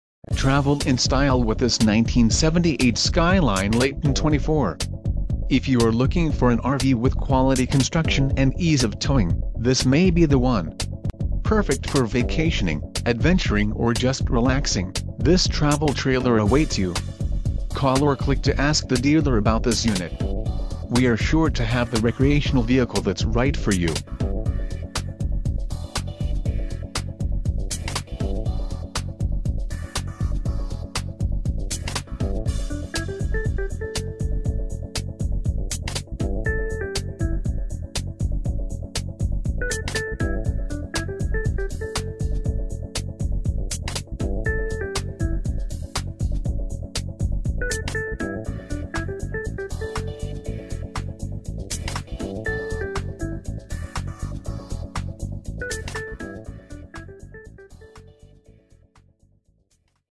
1978 Skyline Layton - Northpoint RV - Chippewa Falls, WI 54
Our lovely Skyline Layton 24 is a classic 1978 model.  This travel trailer has 30 Amp Electrical Service, a Fire Extinguisher, and a LP Gas Furnace.  Beautiful woodwork is found throughout this long lasting model along with retro styling that is easy to appreciate.  The comfortable accommodations include a sleeping are, bathroom with medicine cabinets, and kitchen area.    , 30 Amp Electrical Service\r\nFire Extinguisher\r\nLP Gas Furnace, Medicine Cabinet